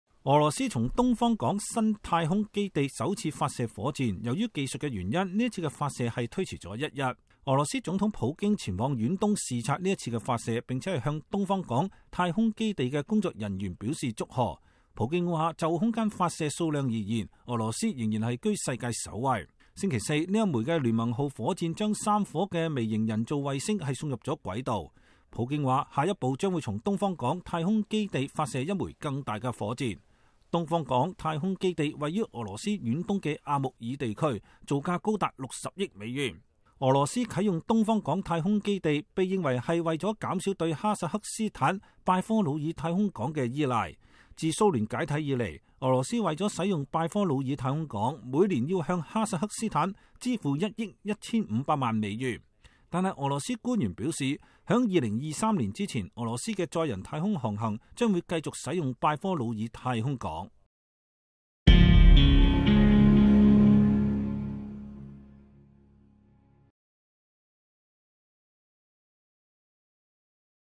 俄羅斯首次從新太空基地發射火箭
俄羅斯從東方港新太空基地首次發射火箭。由於技術原因，此次發射推遲了一天。俄羅斯總統普京前往遠東視察此次發射，並向東方港太空基地的工作人員表示祝賀。他說，就空間發射數量而言，俄羅斯仍居世界首位。星期四，這枚聯盟號火箭將3顆微型人造衛星送入軌道。普京說，下一步將從東方港太空基地發射一枚更大的火箭。